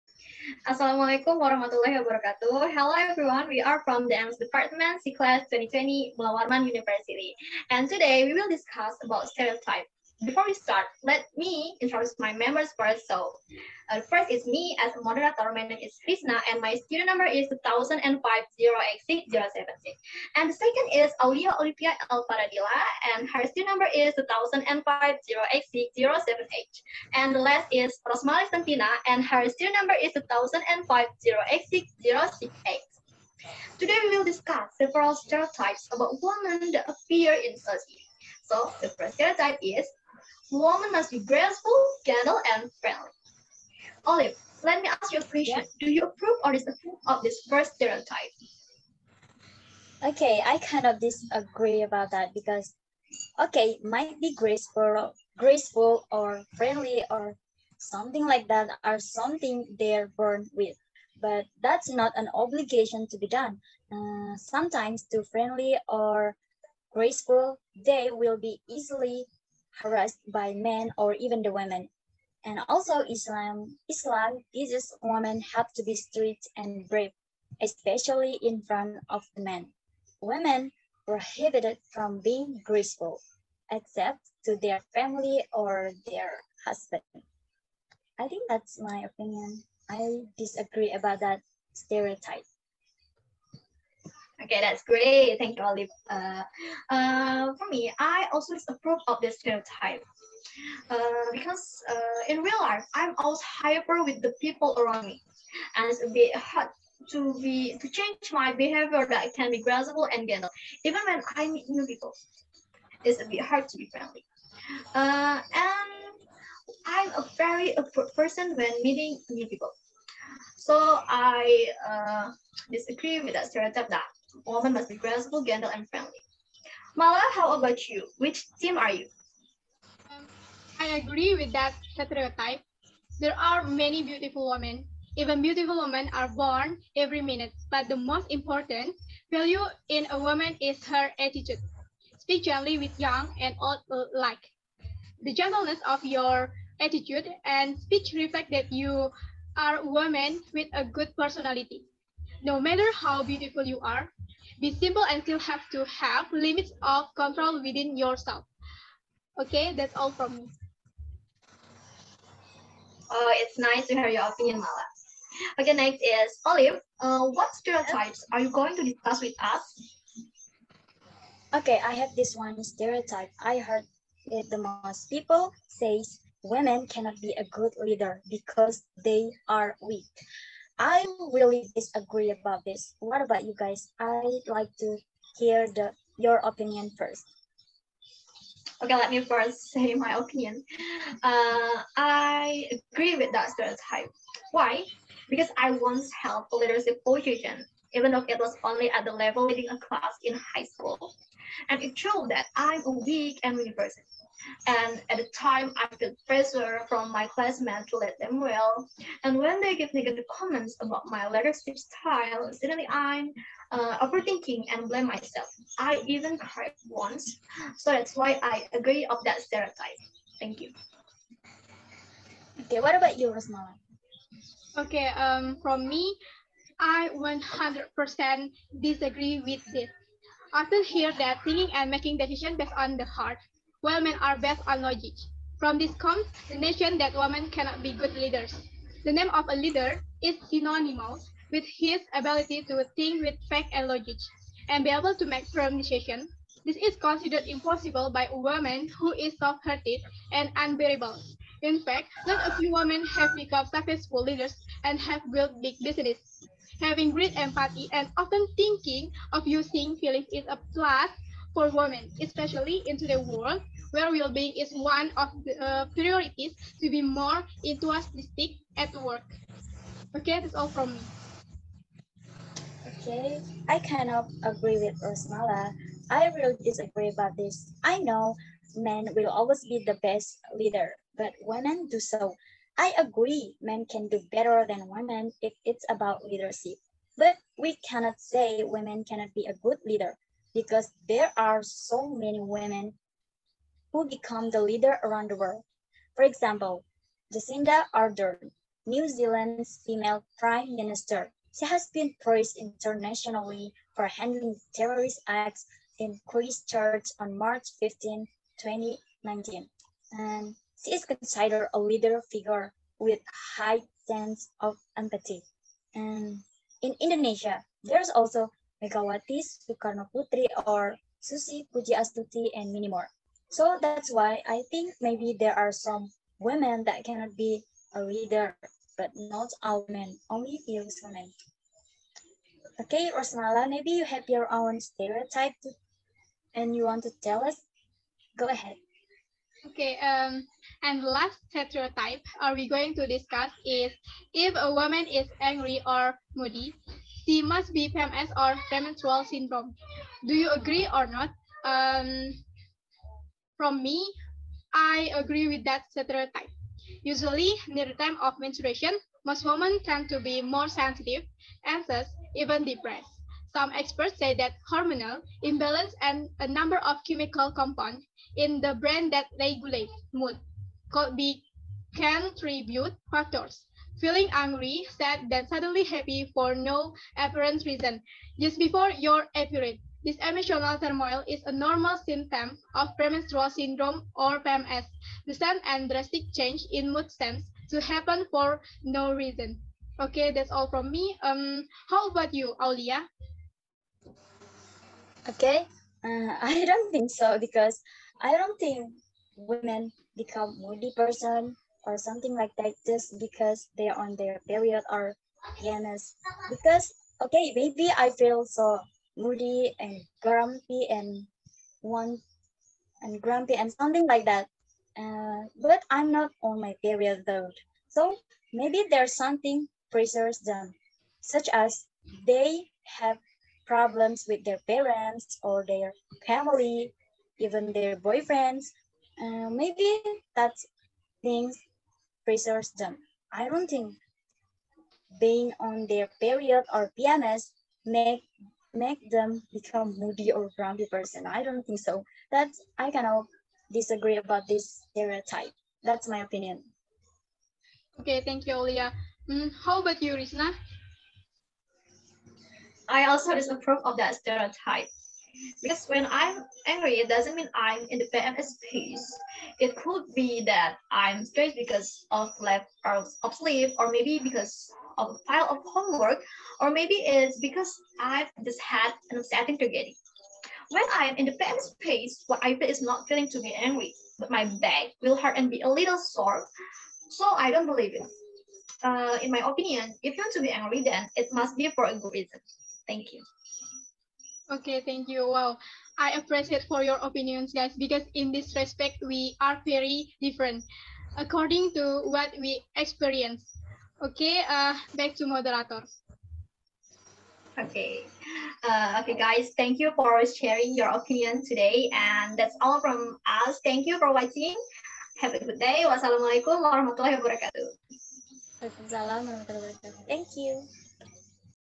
Stereotypes of Women that Usually appear in society
Hello everyone!
We are from The English Department,
C Class-2020, Mulawarman University.
Our group name is "Stereotypes of Women."
And here are the members:
1. Aulia Olivia El Paradila (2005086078)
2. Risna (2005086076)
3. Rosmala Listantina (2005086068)